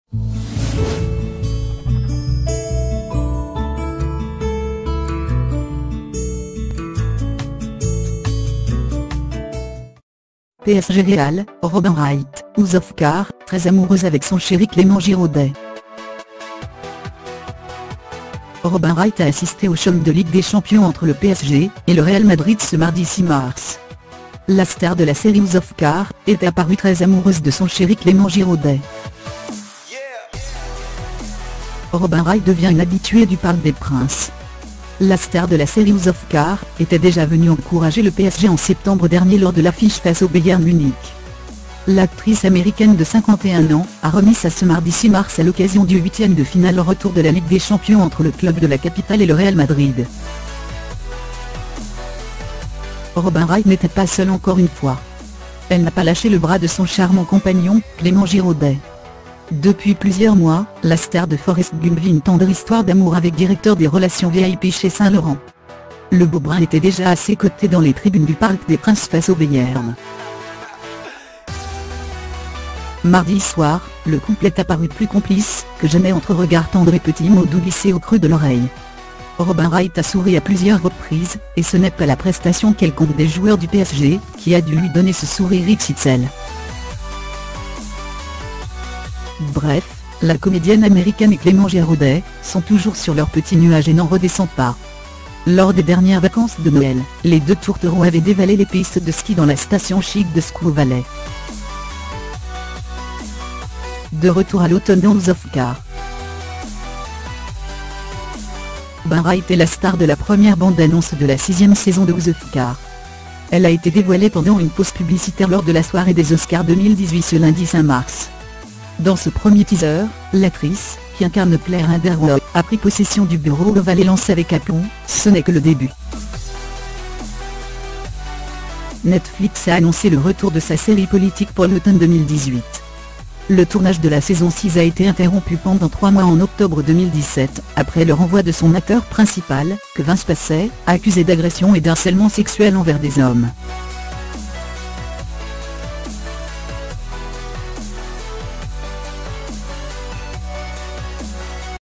PSG-Real : Robin Wright (House of Cards) très amoureuse avec son chéri Clément Giraudet
Robin Wright a assisté au choc de Ligue des champions entre le PSG et le Real Madrid ce mardi 6 mars. La star de la série House of Cards est apparue très amoureuse de son chéri Clément Giraudet.